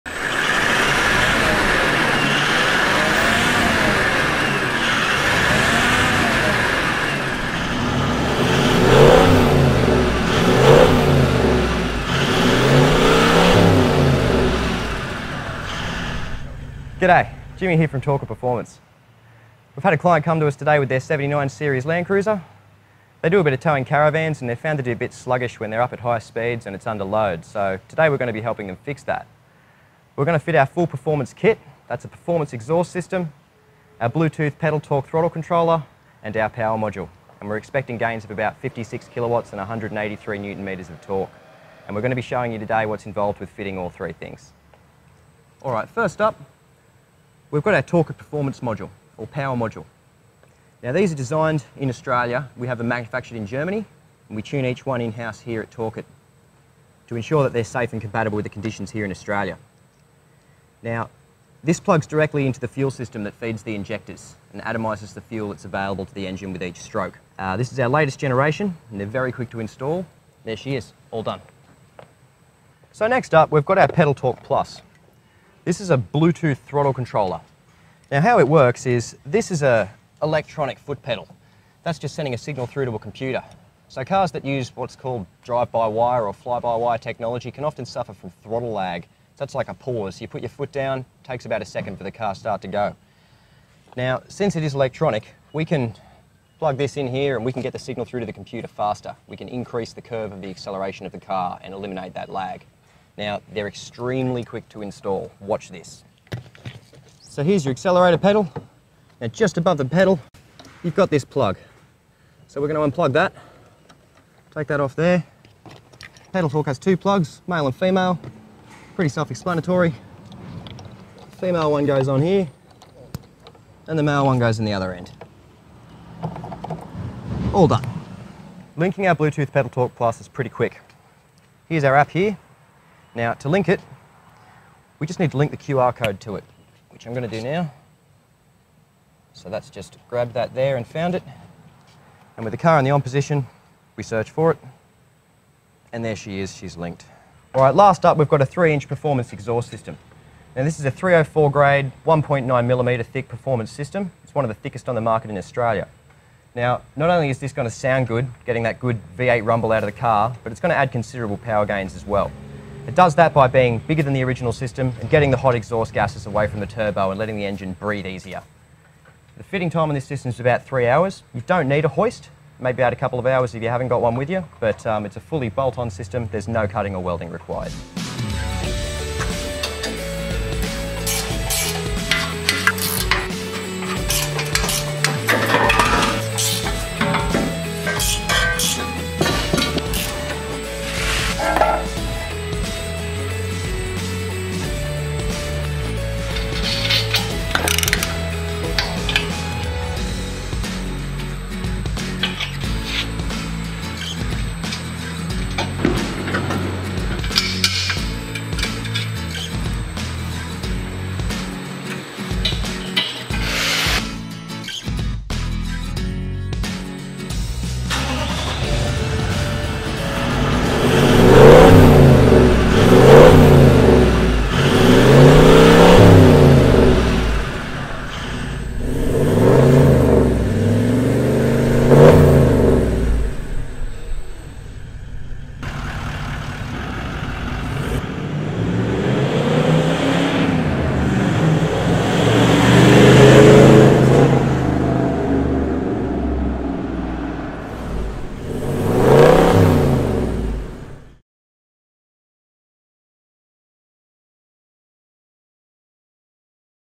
Torqit Performance Upgrade for Landcruiser 79 Series: Exhausts, Power Module, Throttle Controller
How to fit a full performance upgrade to a 79 Series Landcruiser and transform it into a beast! 🐅

This Torqit kit fitted to 79 Series Cruiser gave an increase of 56KW and 183NM.

The 79 Series Toyota Landcruiser was fitted with:

Performance Exhaust
- The thickest & highest grade stainless steel exhaust available
- Internal & External welding for a stronger system
-  High flowing & low thermal conductivity

Power Module
- Increase power, torque & fuel efficiency
- Vehicle specific programming with safety parameters (cannot harm your vehicle's engine)

Pedal Torq Plus
- Australia's first Bluetooth throttle controller
- Increase throttle response; which means you'll be quicker off the mark

All Four x 4 Spares is a Proud Stockist of all Torqit Products

and